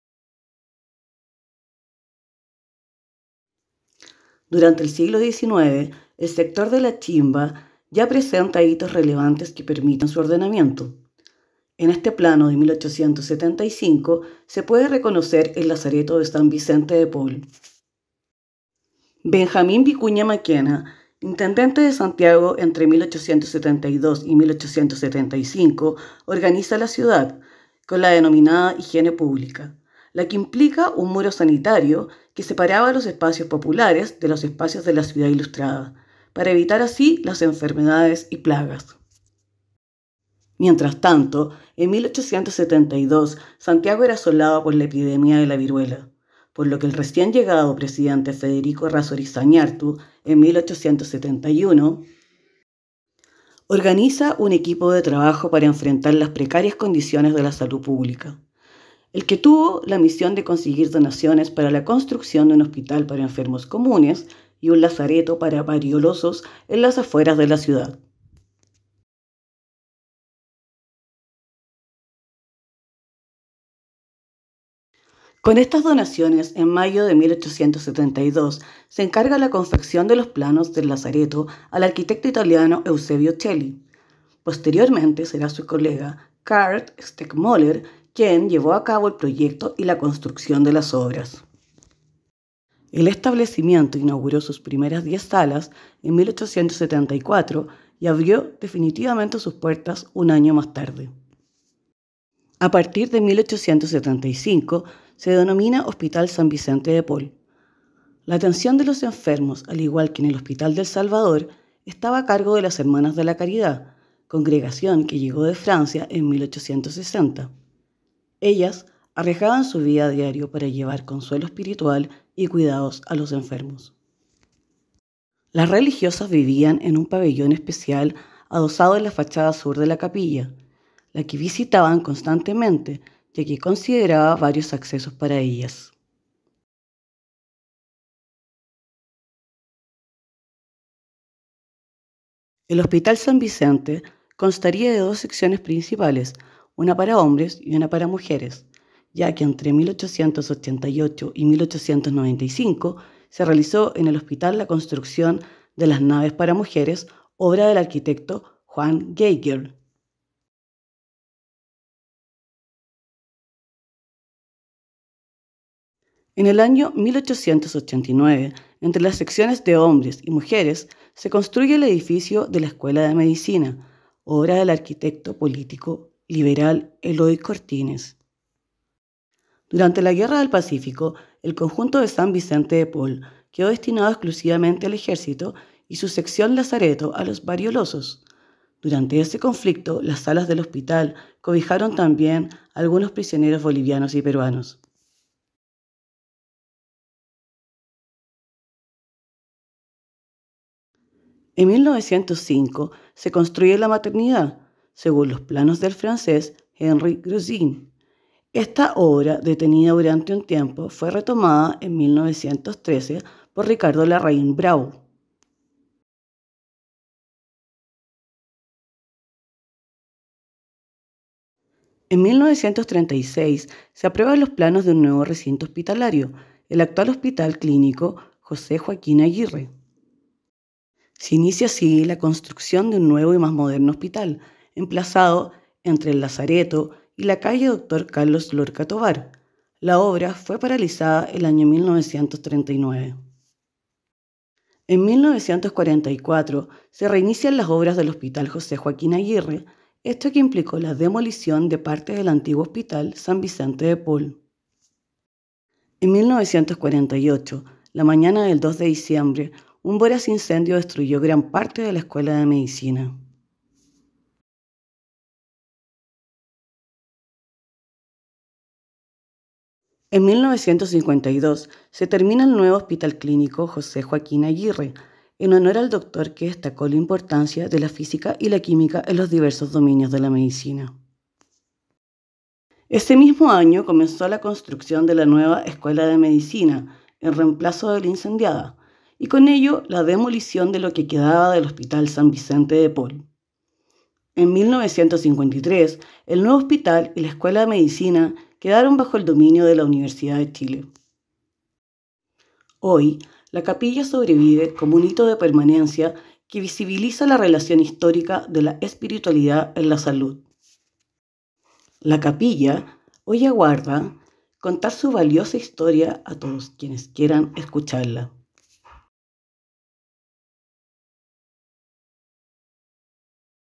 _Capilla San Vicente de Paul : Hito de permanencia y evolución entorno sanitario
En 1872 Santiago era asolado por epidemia de viruela. Se recaudan fondos para construir un recinto sanitario. En  1874 abre la sección de hombres del Lazareto, que considera al centro una Capilla y adosado a esta, un edificio donde vivían las Hermanas de la Caridad, congregación francesa que se hace cargo de este recinto.
La sección de mujeres comienza su construcción en 1888 y culmina en 1895. En  1889, entre estas secciones  se construye la antigua Escuela  de Medicina y en 1905 se comienza a construir la maternidad.
Para 1936 se aprueban los planos para el nuevo Hospital, el actual José Joaquín Aguirre. En 1948 la antigua Escuela de Medicina sufre un voraz incendio, quedando destruida.
Ya en 1952 se termina el nuevo Hospital Clínico de la Universidad de Chile y comenzó la construcción de la nueva Escuela de Medicina, la que culmina en 1954.
La capilla persiste como hito de permanencia y testigo a todos los cambios acontecidos